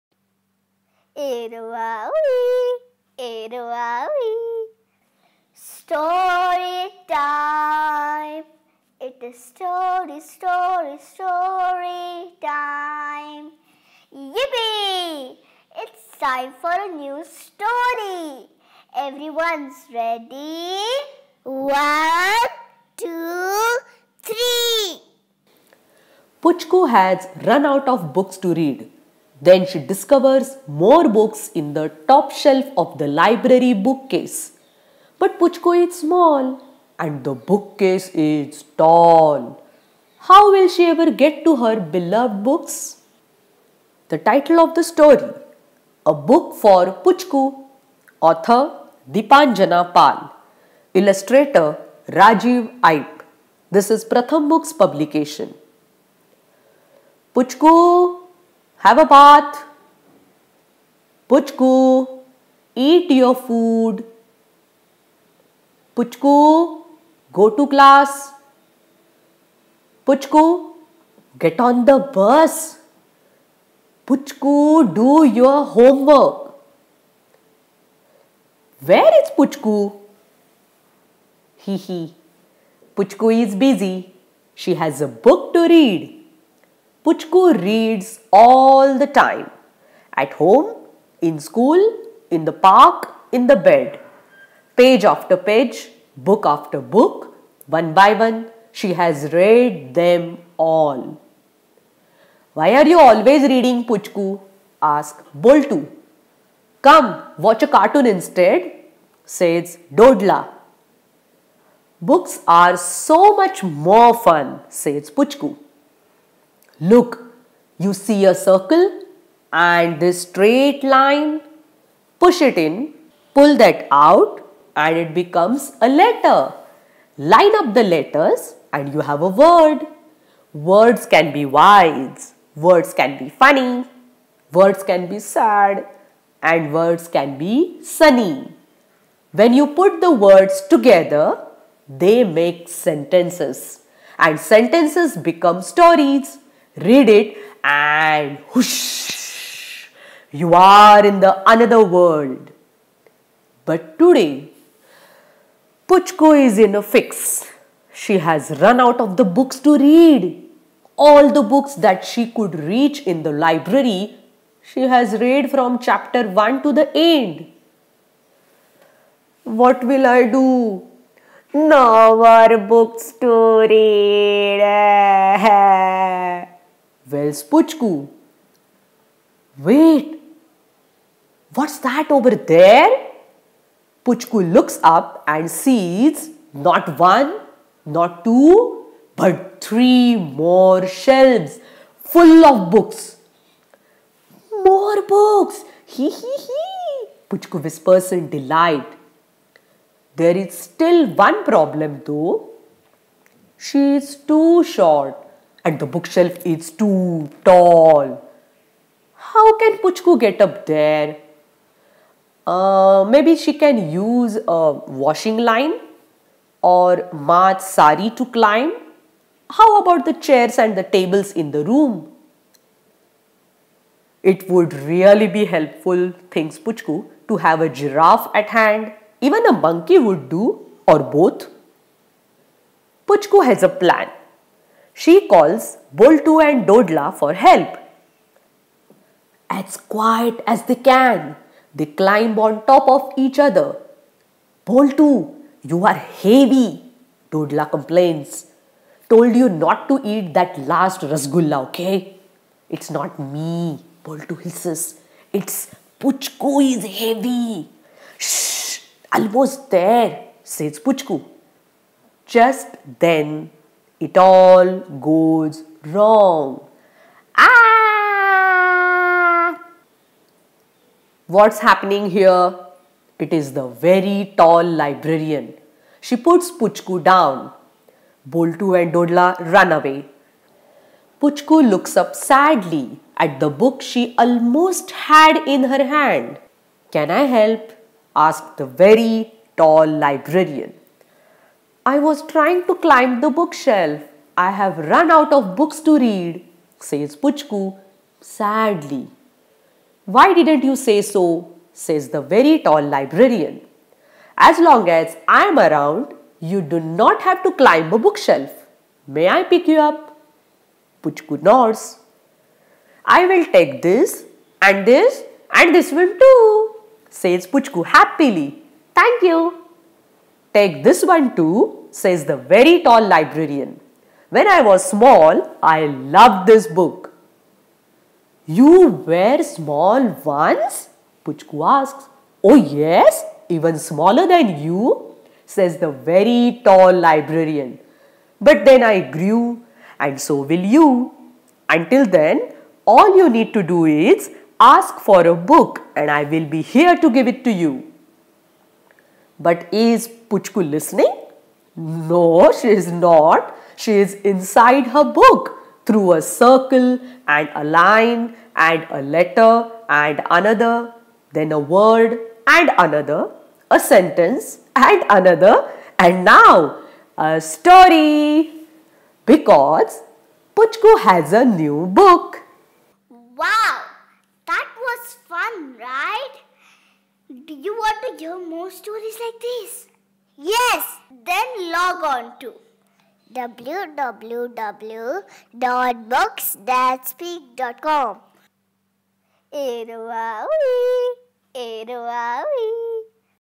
English stories for kids - A Book for Puchku - Pratham Books
Puchku has run out of books to read. Then she discovers more books in the top shelf of the library bookcase. But Puchku is small, and the bookcase tall. How will she ever get to her beloved books?

Story written by Deepanjana Pal, illustrated by brilliant Rajiv Eipe, published by Pratham Books. Narrated by Asawari Doshi.

More about Books that speak-